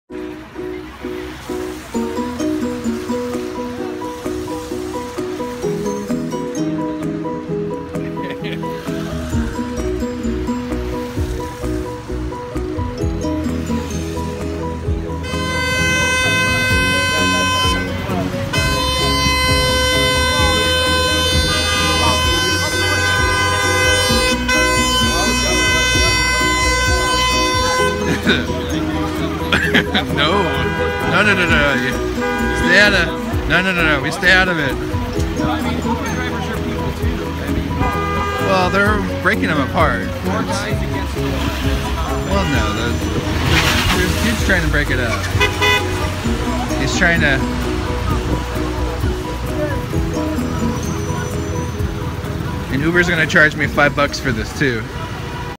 Uber Driver Fight
My Uber driver getting in a fight with a bicyclist. Great last night in Paris.